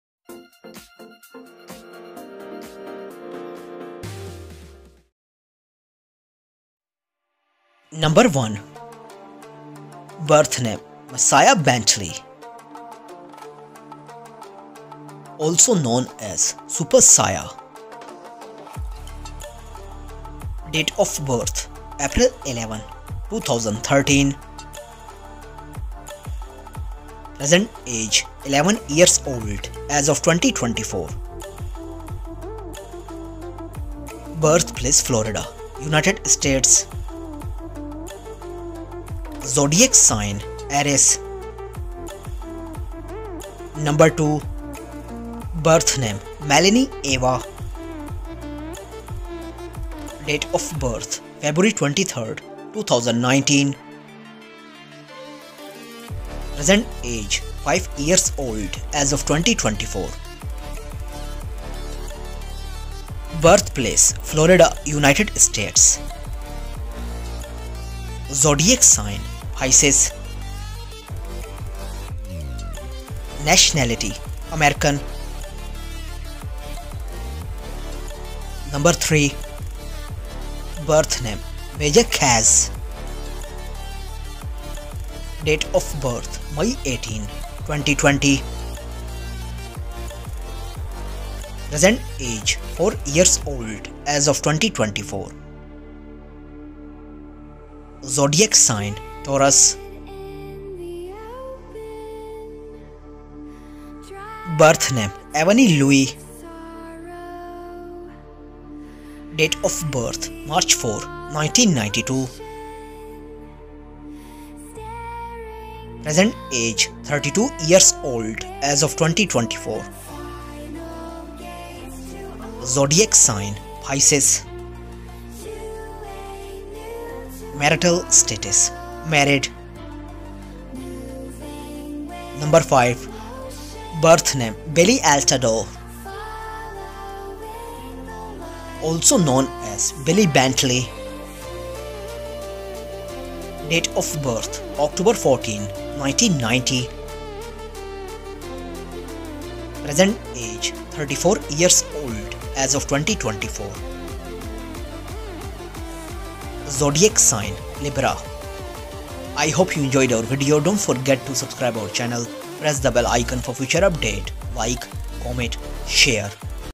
Beam SQUAD Members Real Name And Ages 2024
(Source Famous Birthday)

Couples channel featuring prank wars which was ran by YouTubers with personal channels called Billy Bentley Vlogs and It'sMe Eva.

In 2017, some of the most watched videos on the channel became "Moving Bear Prank On 3yr Old" and "Alone Prank On 3yr Old Son."